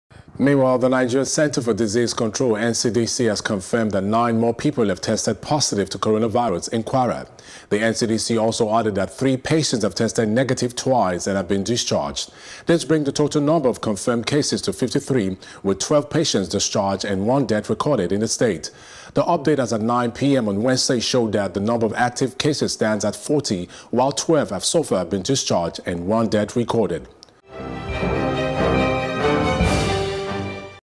Kwara Records Nine New Cases Of COVID-19
The Nigeria Centre for Disease Control (NCDC) has confirmed that nine more people have tested positive to Coronavirus in Kwara.

The NCDC also added that three patients have tested negative twice and have been discharged.